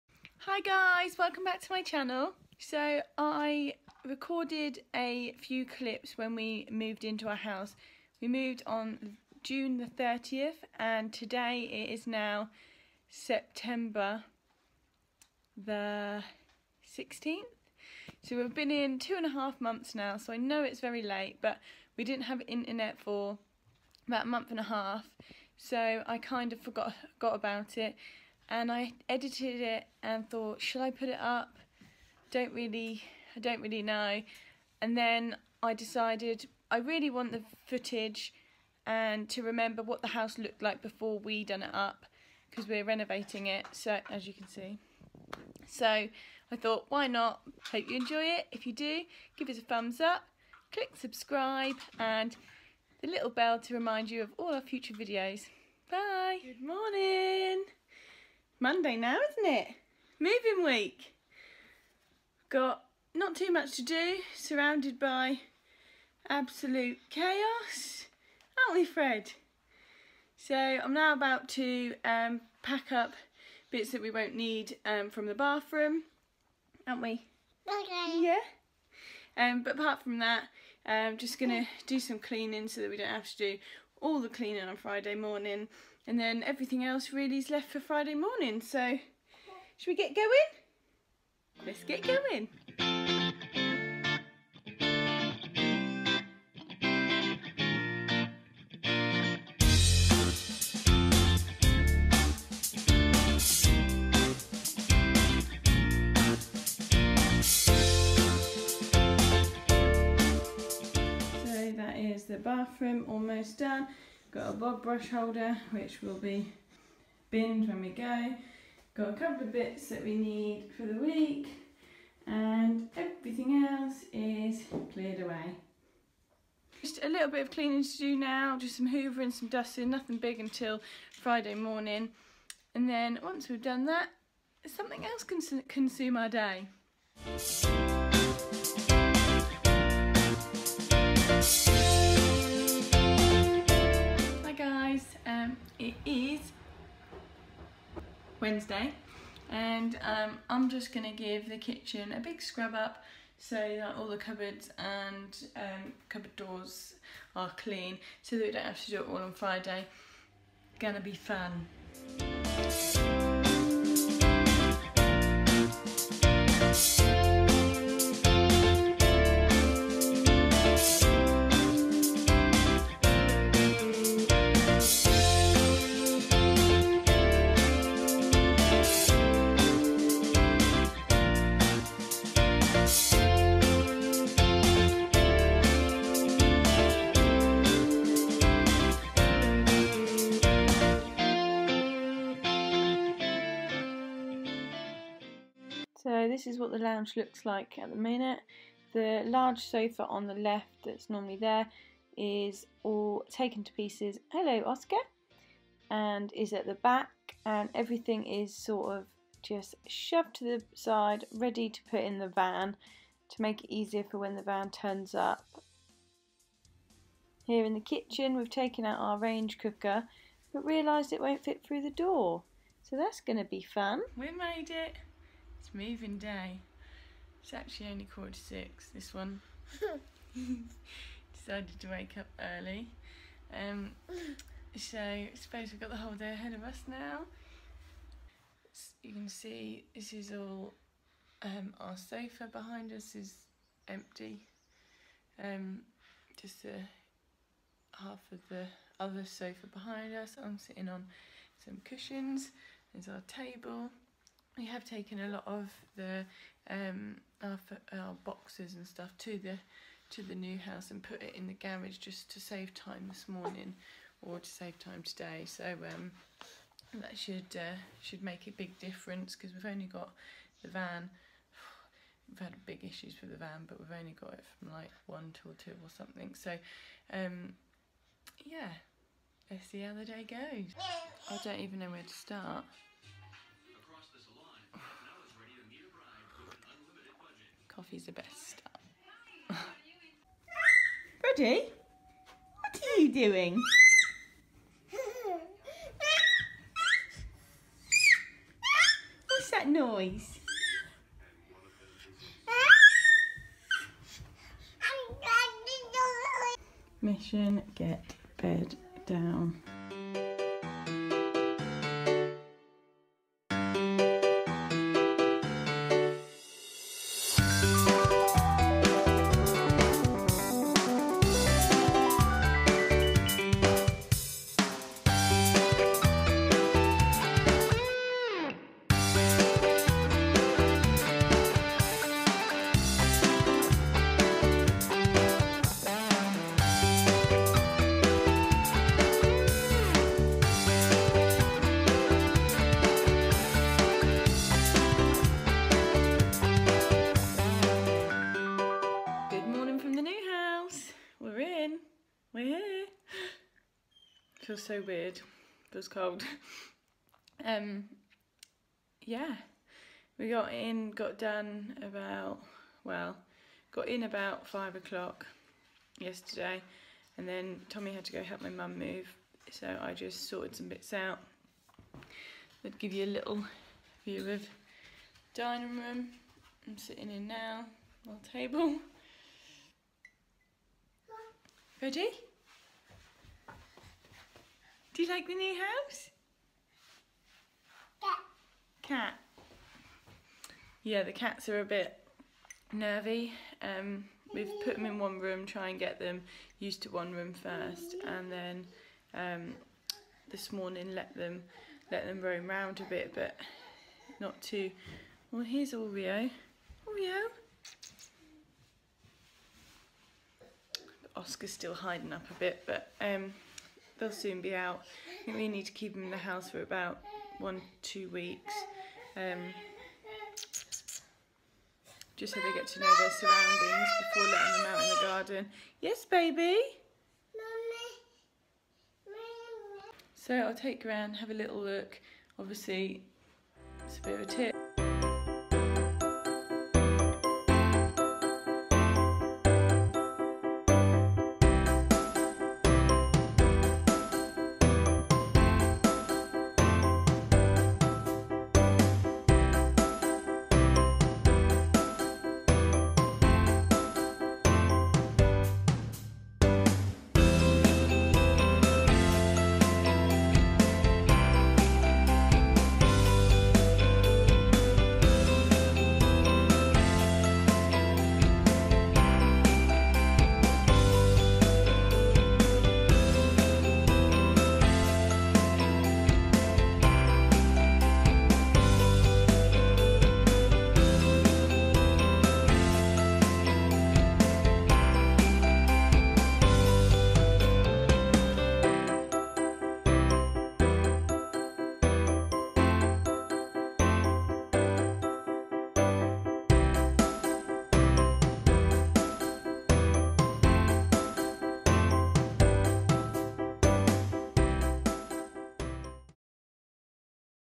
Weekly Vlog | Moving home | New house tour
Hey Guys, i got a few clips of moving week so thought it put them together to remember how the old and new house looked.